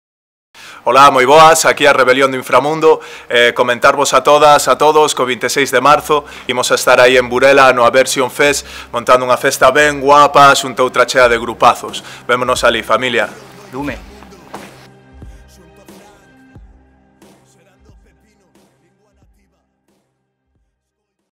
Aversion Fest 2016 - Saúdo Rebeliom do Inframundo
Rebeliom do Inframundo mandounos un saúdo en vídeo co motivo da súa participación no Aversion Fest 2016!!